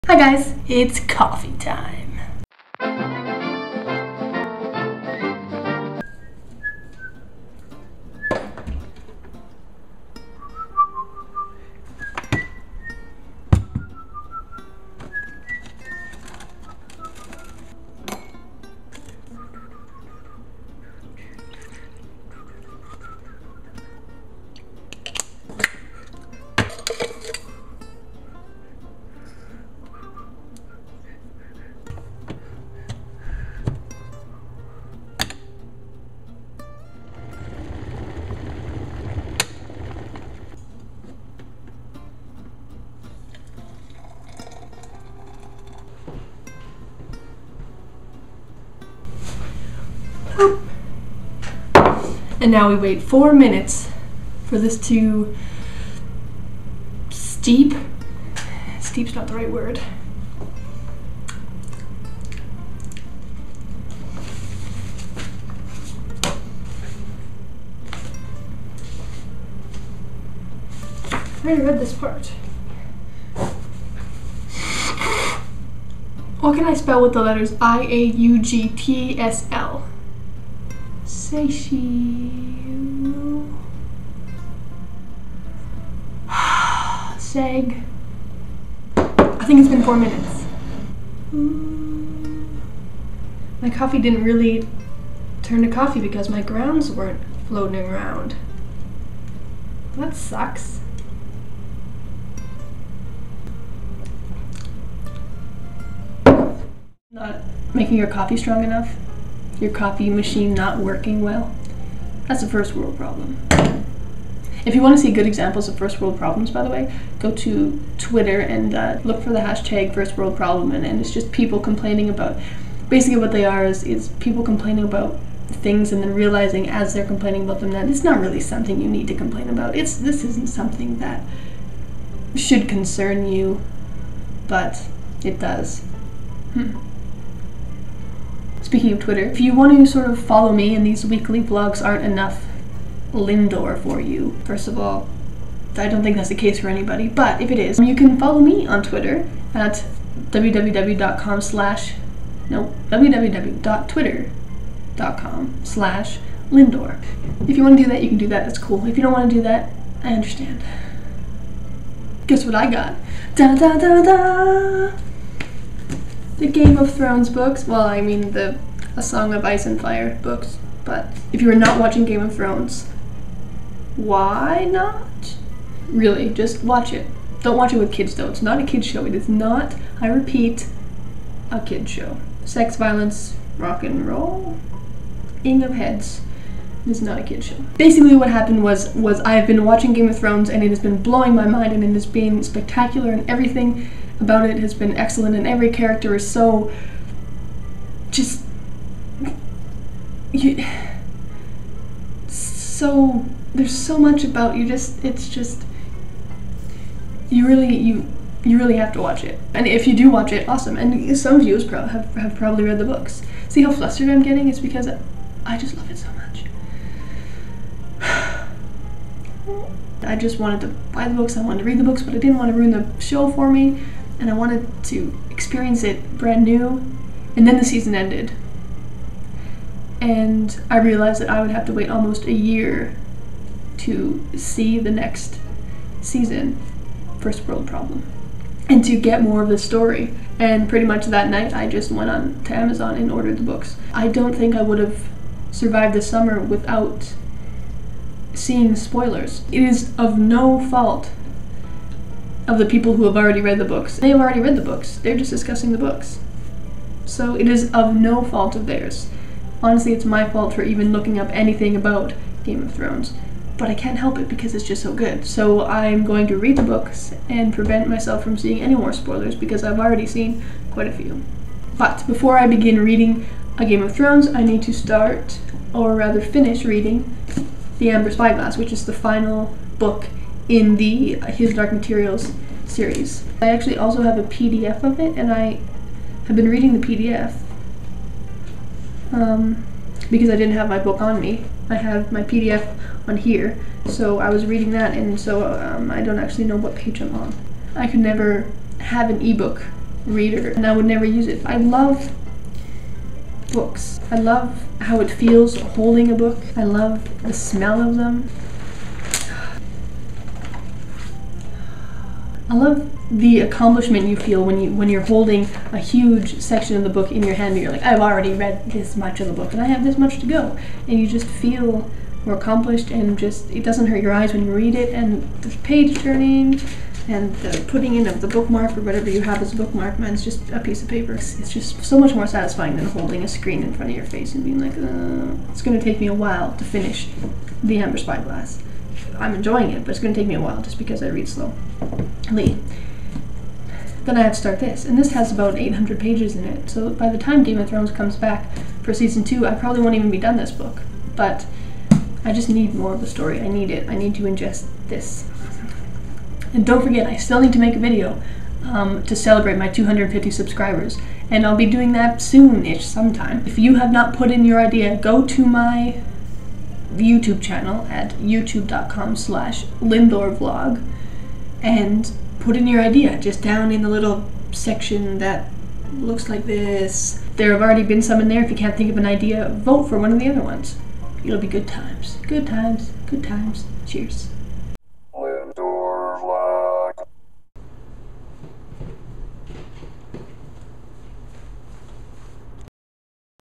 A Vlog of Coffee and Real Books
I make coffee, talk about my Game of Thrones obsession, and my affinity for real books over eBooks!